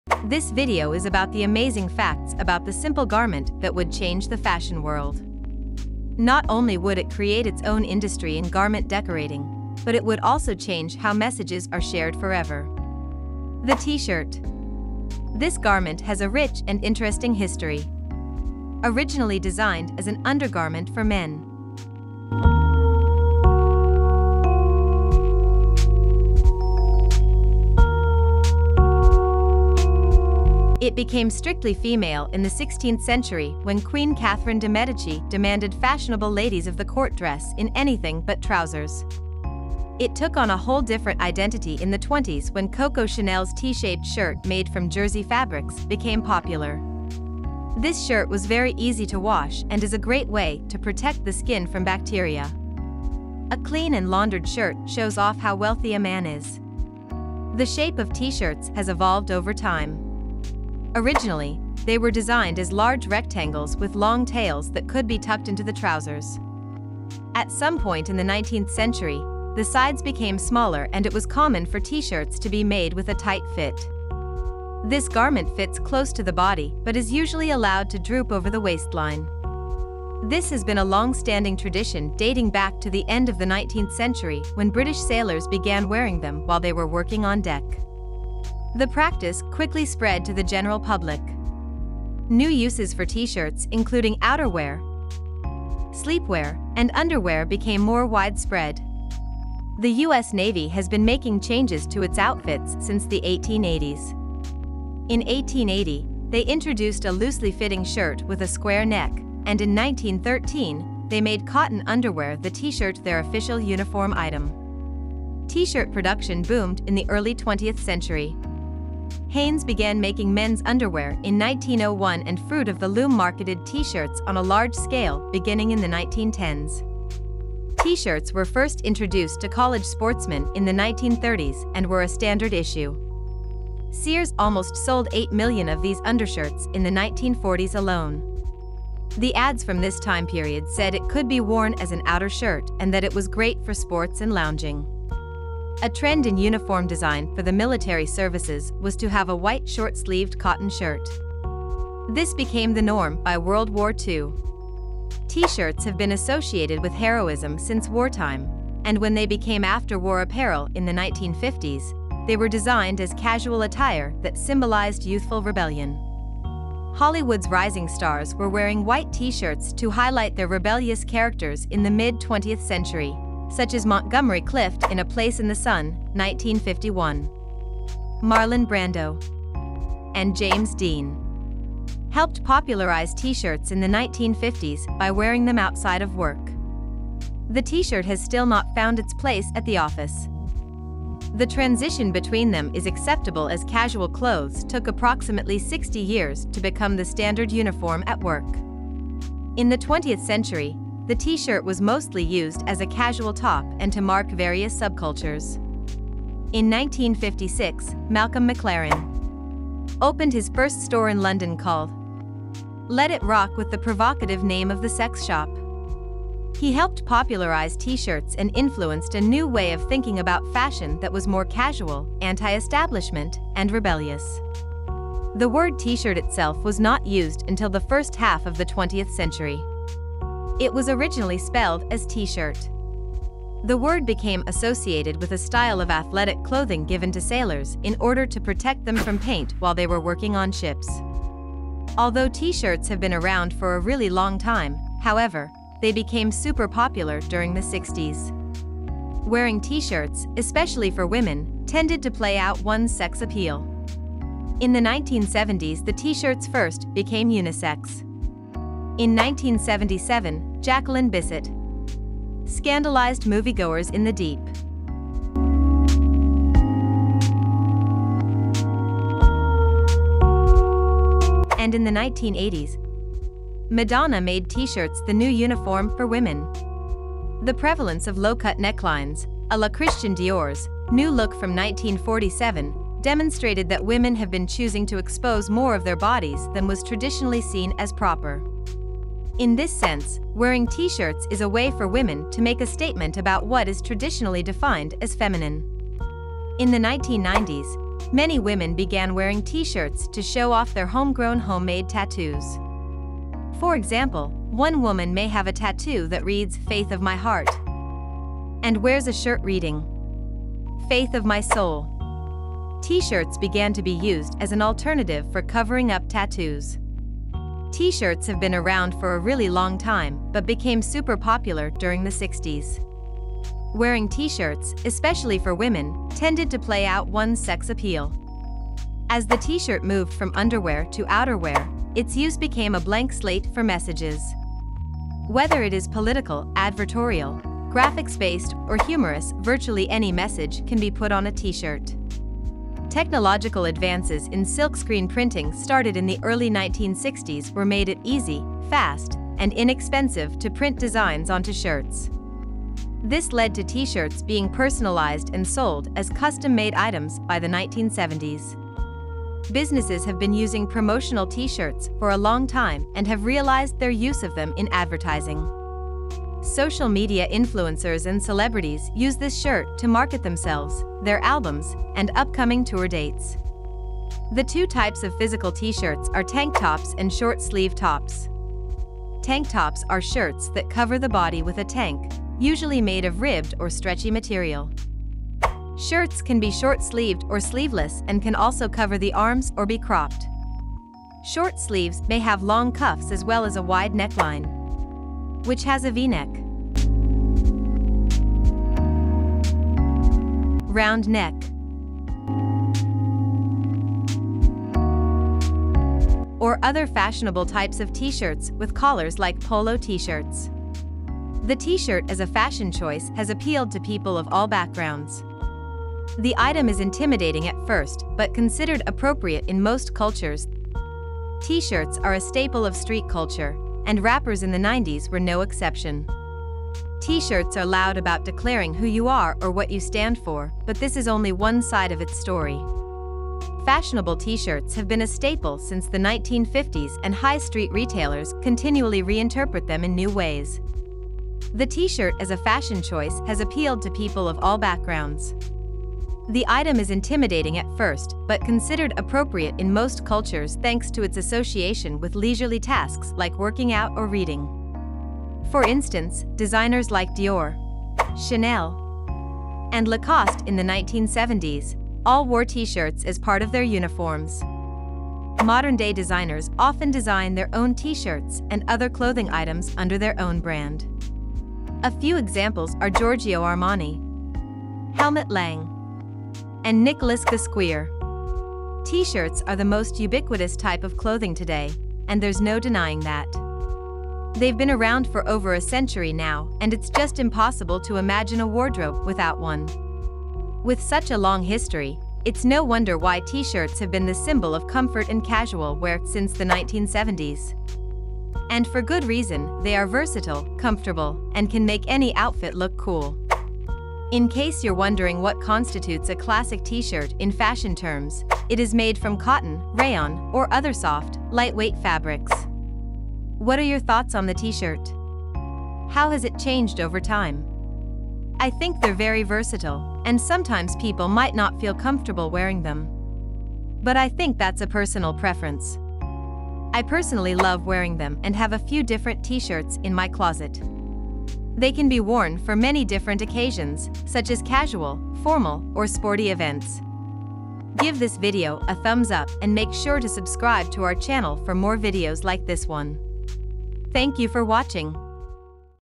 The Surprising Evolution of the T-Shirt
The t-shirt is a garment that is meant to be worn on the upper body and is usually made of woven cloth. The shirt is often made of cotton, but can also be made of wool, silk, polyester, or any combination thereof. This video is a great way to learn about the evolution of the t-shirt and where it came from.